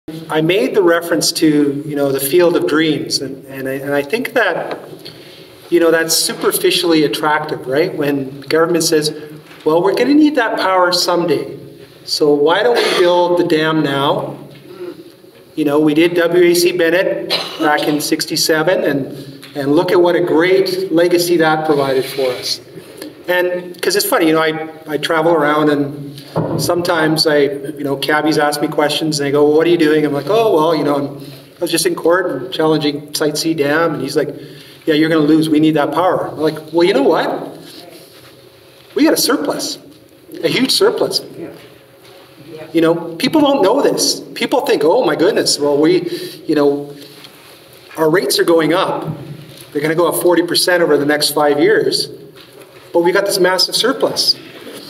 BC Hydro rates are going up!
Lawyer John Gailus points out the anomaly facing ratepayers.